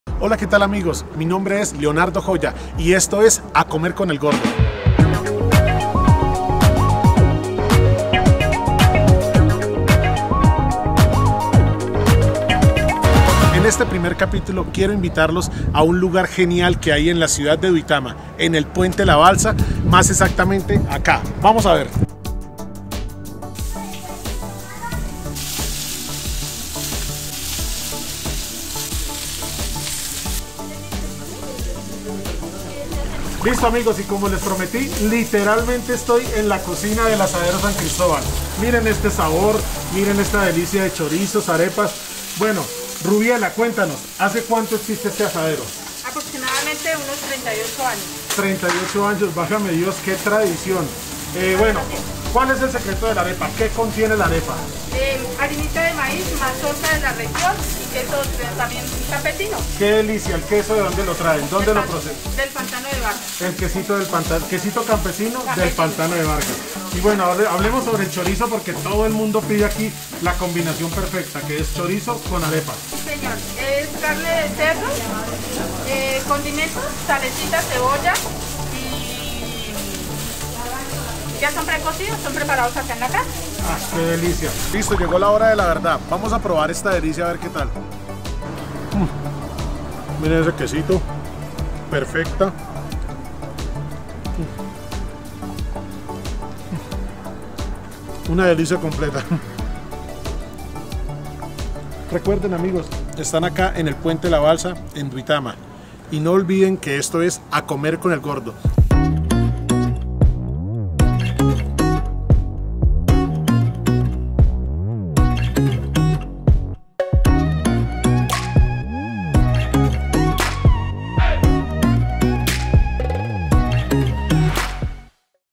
Arepa Boyacense
Un tesoro gastronómico de nuestra Boyacá con una tradición de 38 años AsaderoSanFrancisco ubicado en la ciudad de Duitama en el puente la balsa